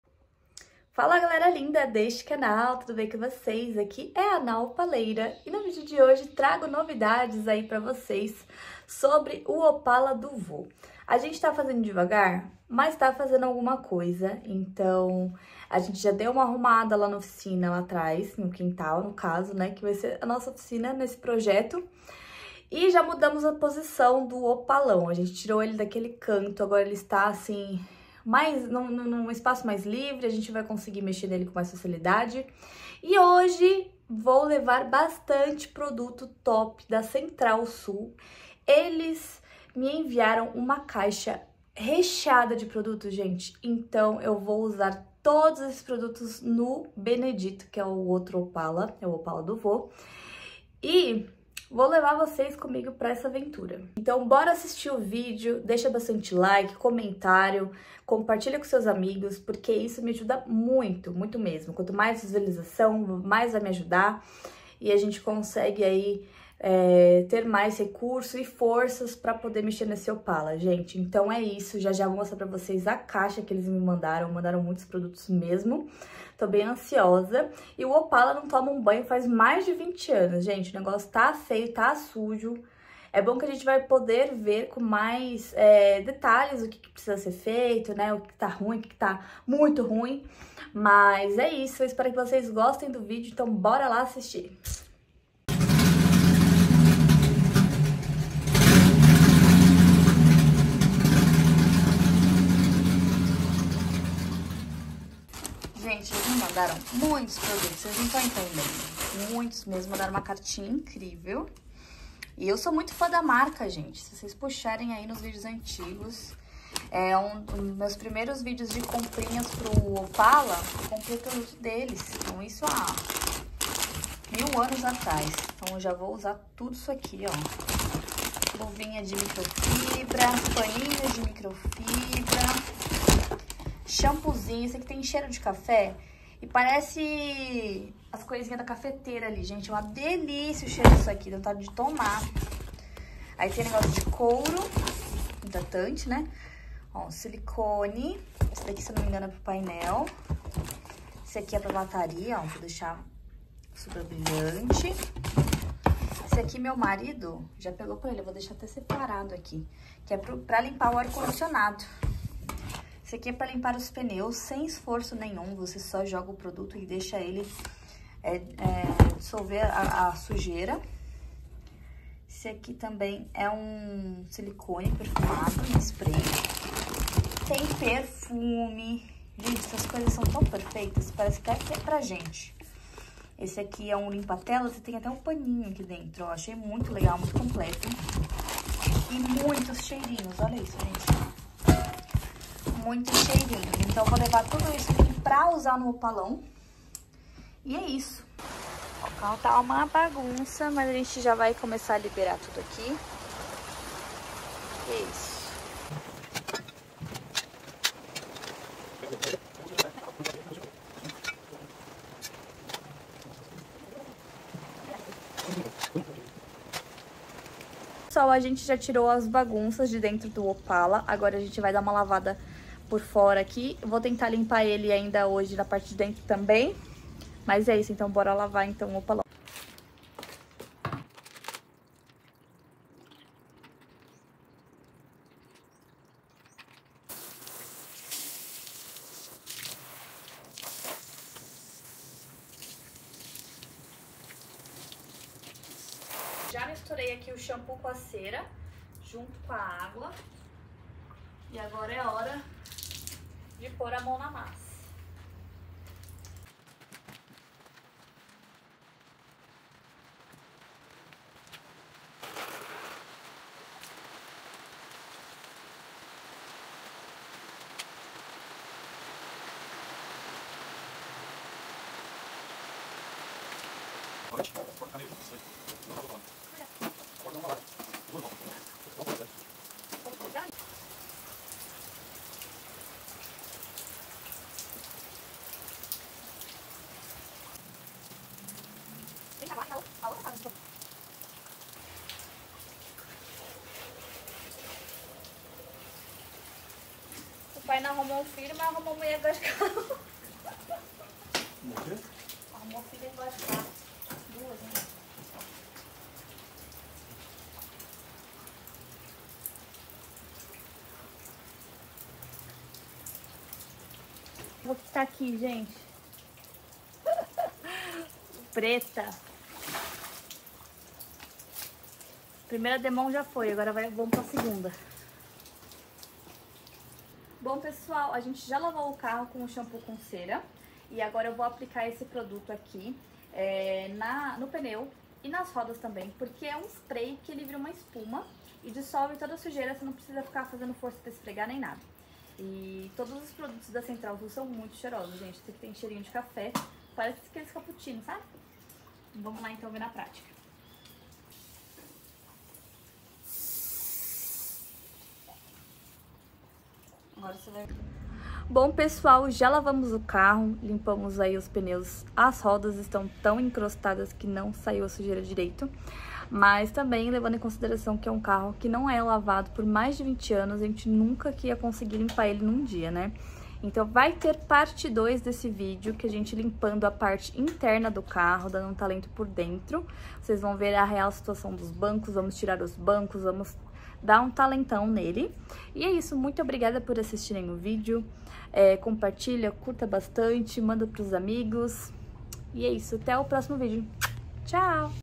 LAVANDO O OPALA BENEDITO APÓS 20 ANOS !!!!! 😱
Fala lindezas deste canal , tudo bom com vocês ?

No vídeo de hoje vou mostrar o banho que o opalao ganhou após tantos anos , aos poucos as coisas vão melhorando !!!
Espero muito que gostem do vídeo ❤️